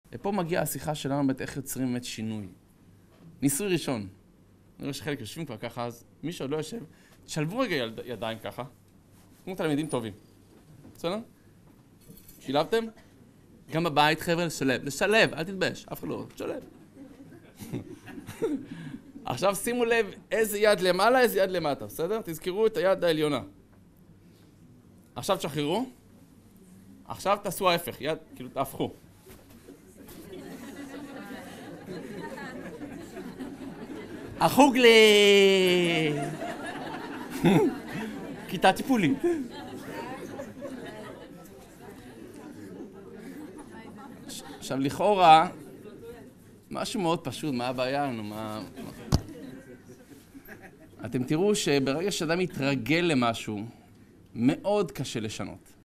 הרב פנגר בכיתה טיפולית
קטע מתוך ההרצאה "שינוי" באתר הידברות